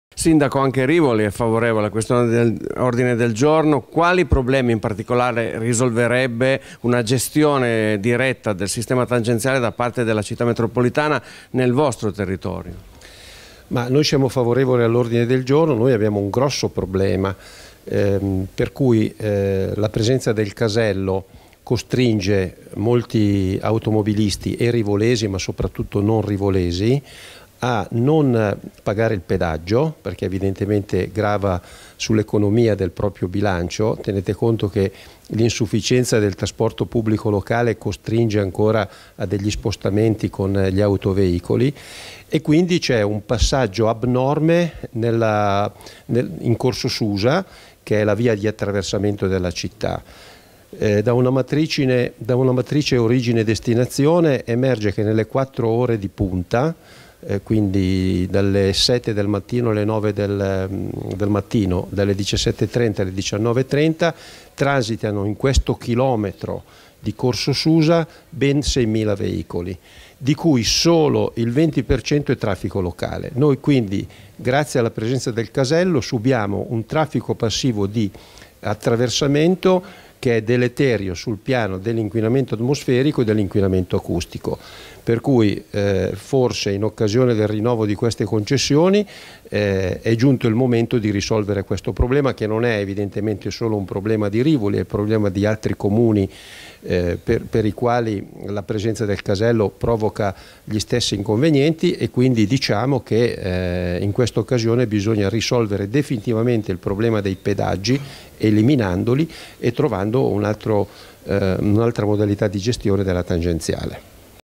Concessione tangenziale di Torino: intervista a Franco Dessì
Il sindaco di Rivoli commenta l'ordine del giorno approvato dalla Conferenza metropolitana.